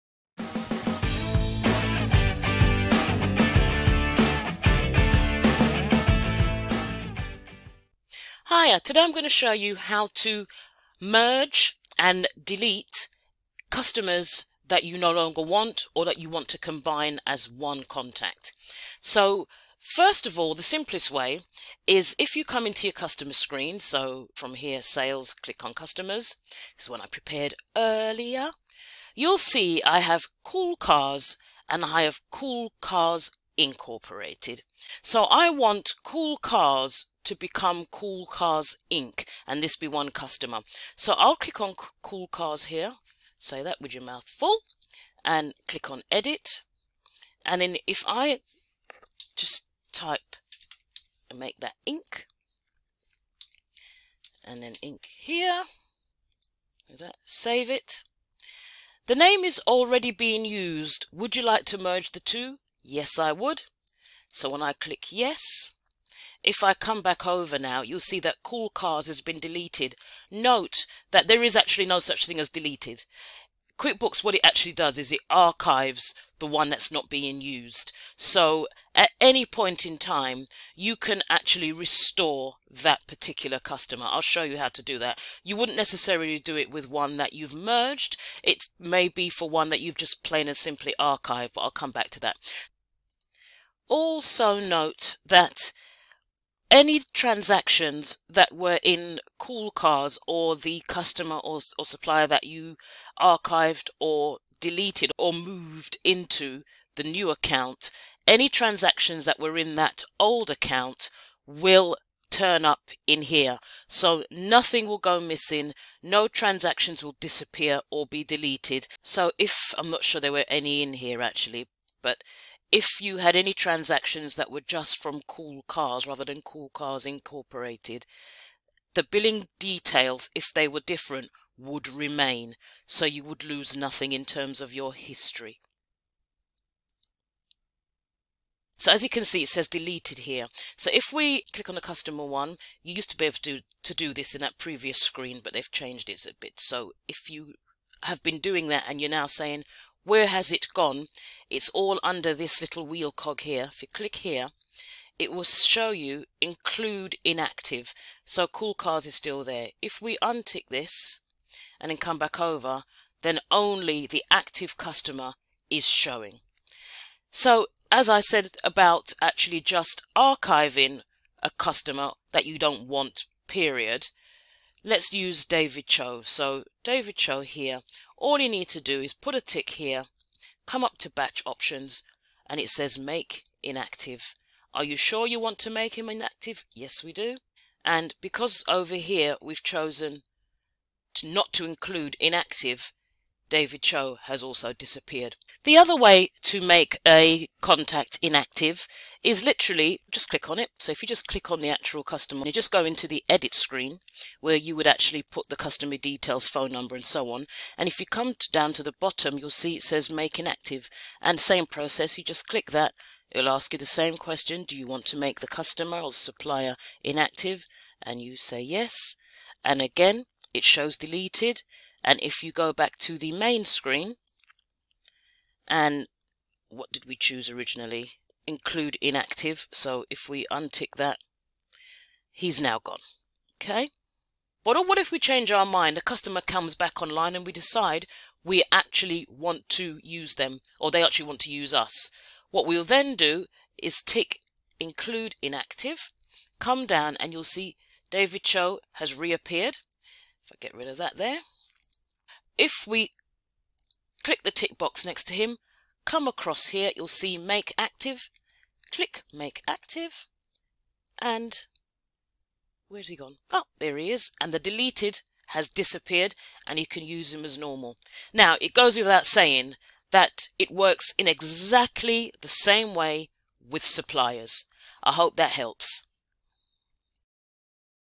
How to Delelte/Merge Duplicate Customers/Suppliers in QuickBooks Online
Learn how to merge and delete duplicate customers and suppliers records in QuickBooks Online, so you ensure they are organised efficiently.

Tools and Resources to make your financial life easier